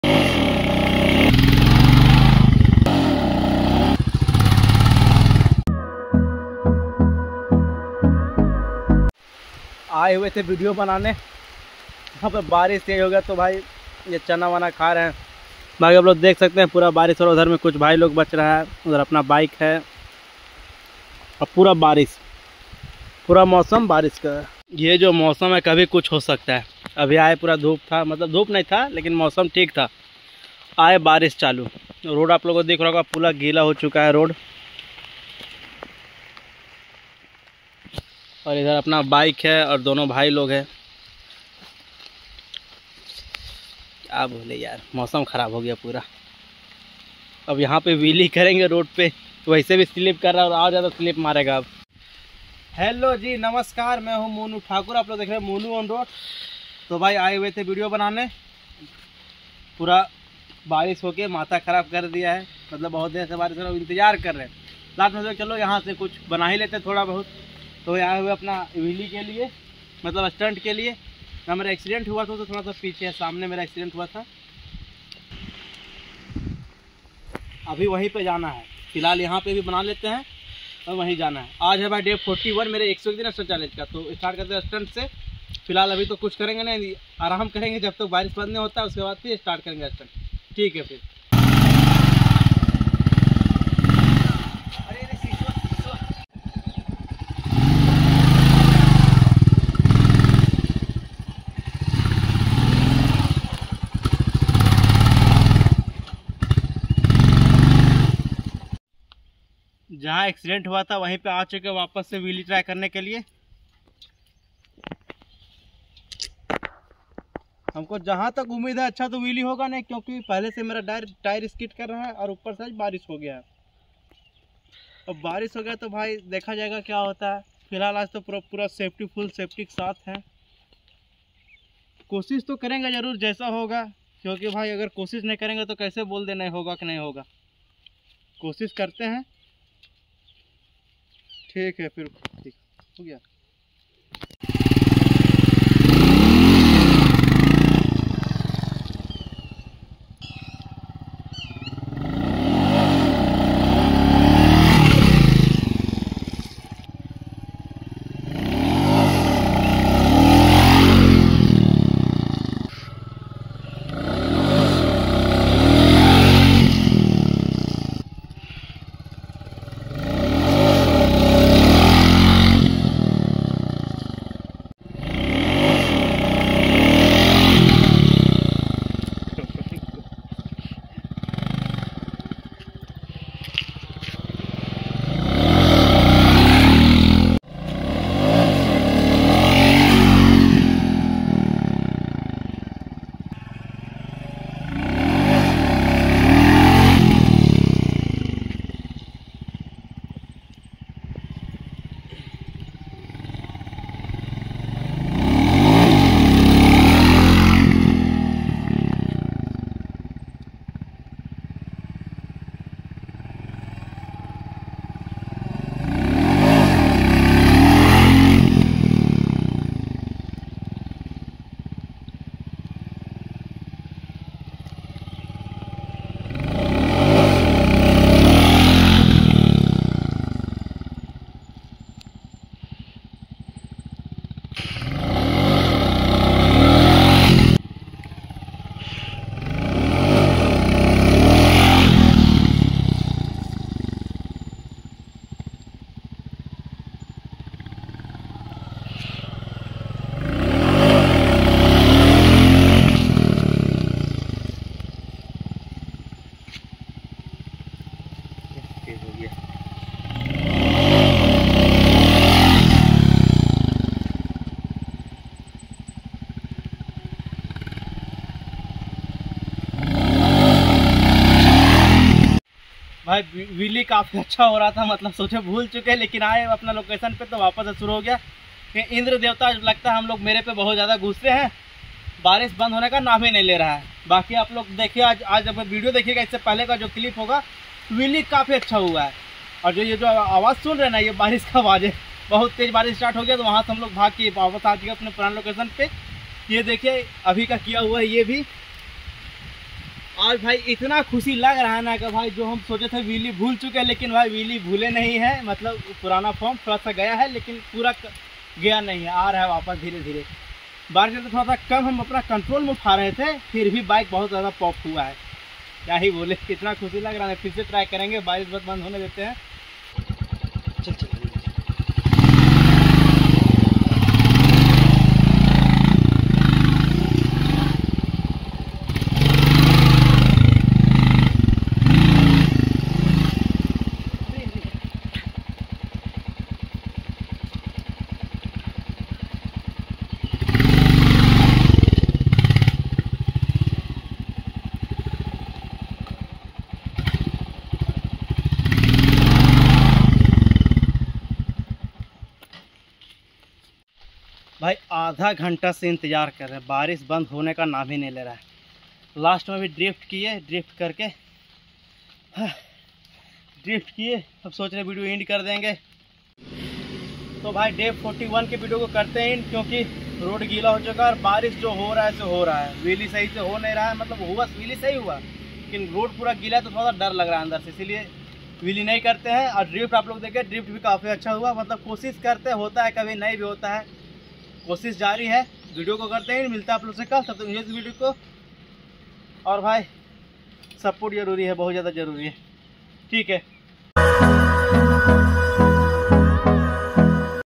Barish Mai Whellie 😱 // Day 41 // 101 Days of Stunts Challenge // stunts challenge // MonuonRoads
Aaj To Bhai Barish mai v Whellie Mar Diya

Kafi Acha Raha Whellie

Aap Log Video Mai Chake Kre Kaisa Raha Whellie

One Life One Dream Travel The World 🏍️🏍️🏍️🏍️🌍🌍🌍🌍

Save Nature Save Mother Earth 🙏🙏🙏🙏

monu_on_roads